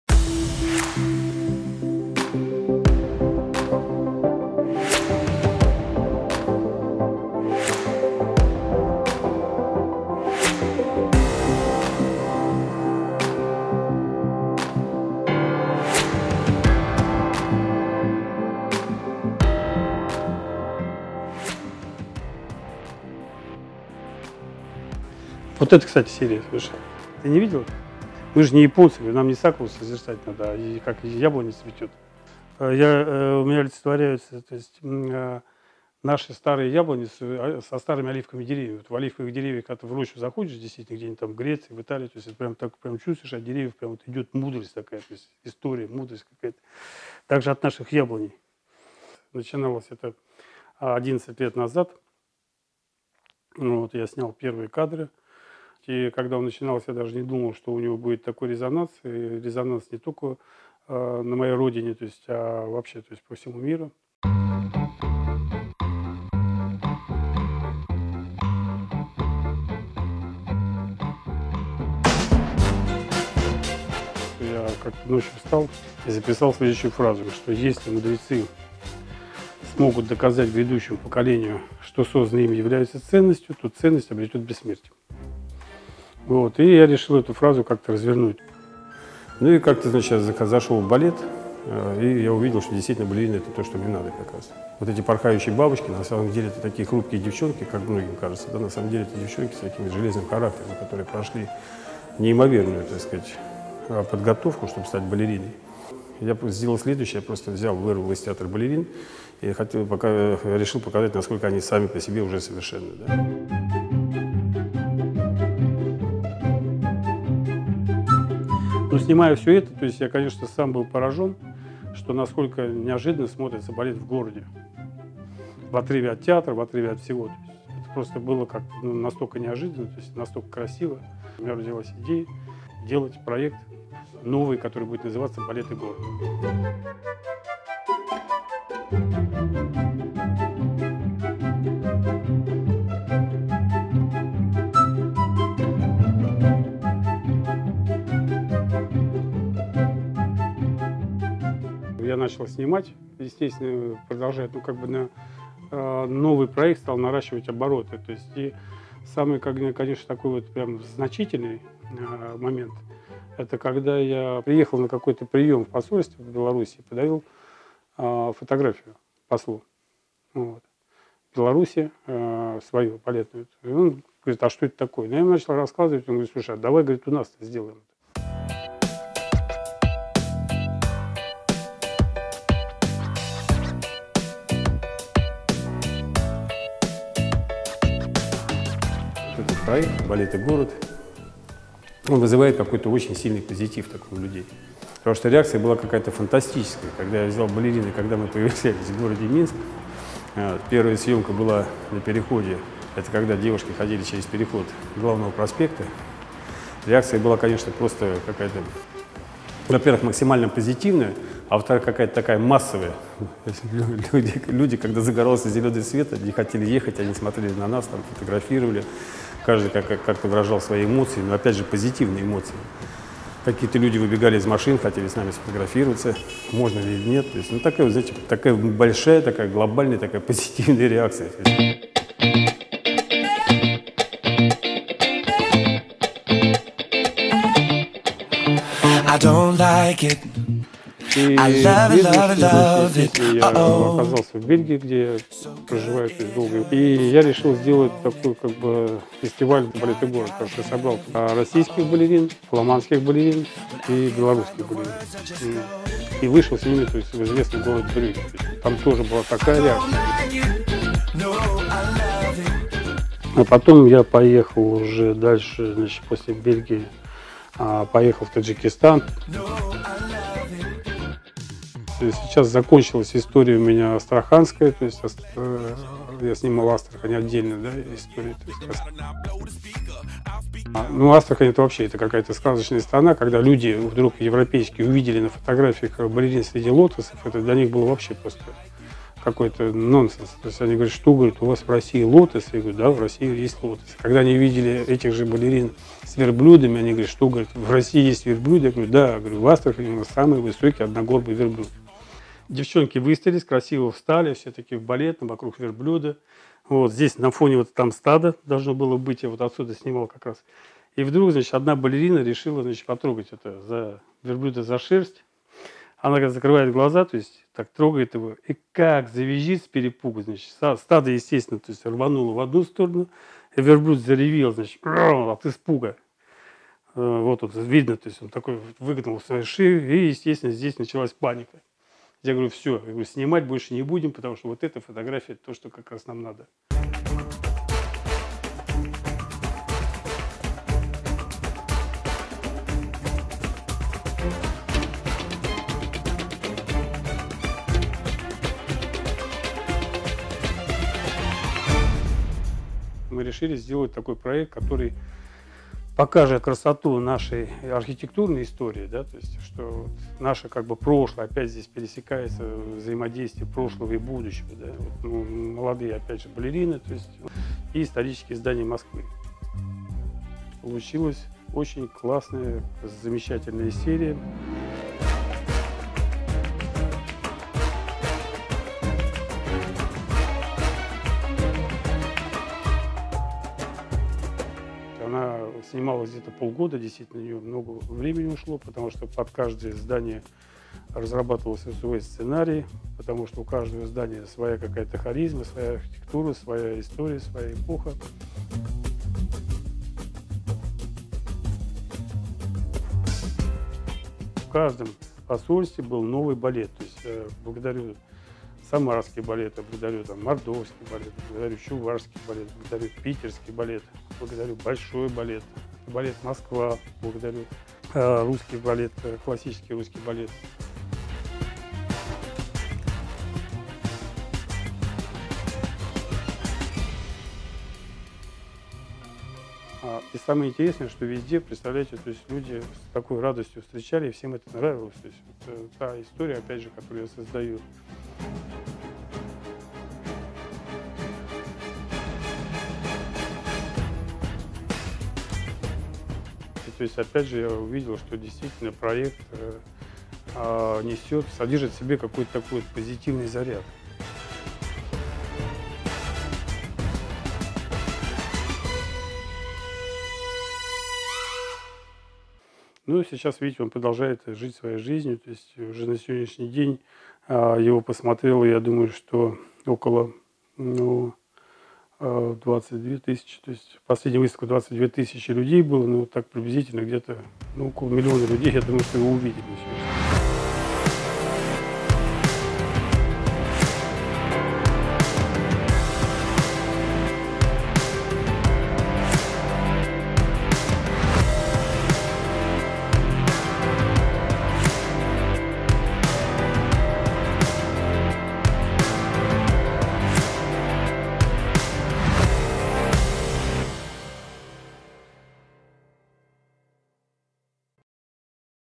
Проект Владимира Чикина Балет и Город - от автора проекта
Владимир Чикин, автор проекта Балет и Город рассказывает о проекте, идее его создания и ходе реализации своих творческих идей. Смотрите!